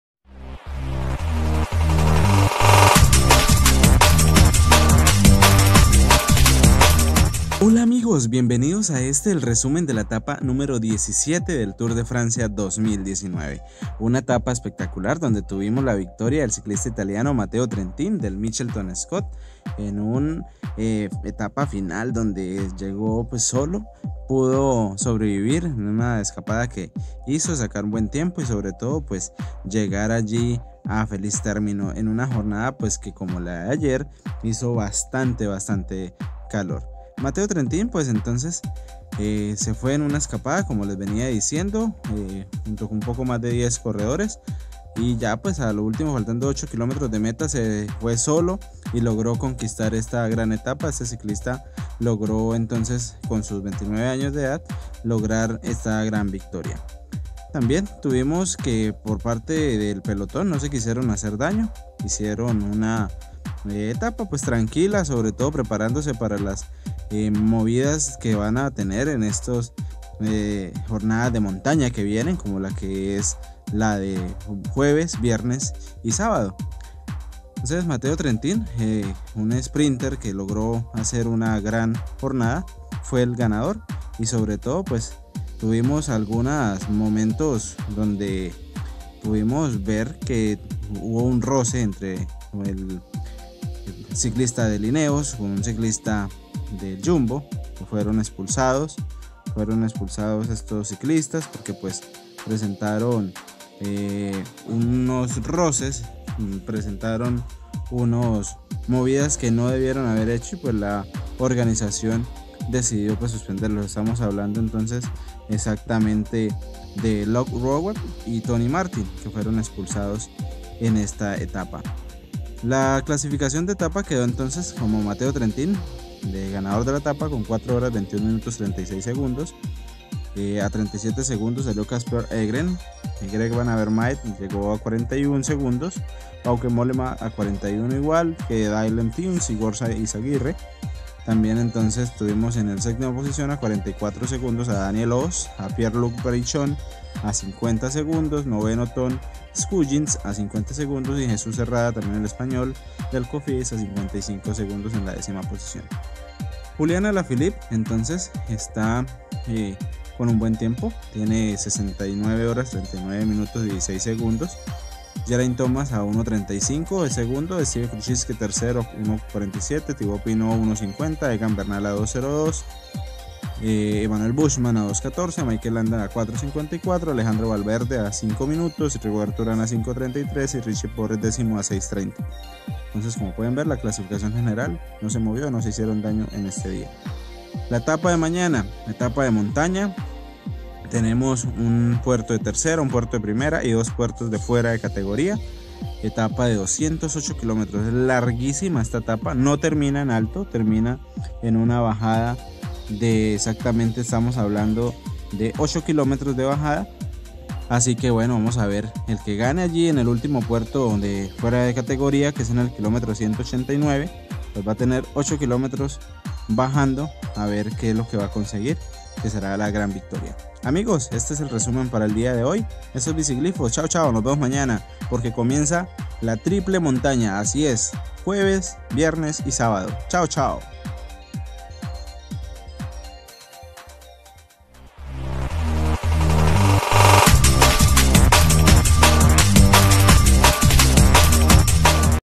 Resúmen ETAPA #17 Tour de Francia 2019
MateoTrentin se llevó la etapa diescisiete del TDF2019, la general no se movió y mañana comienza la Triple Montaña donde se definirán los tres primeros lugares del podio final.

⬆️Y mantente conectado al ciclismo⬆️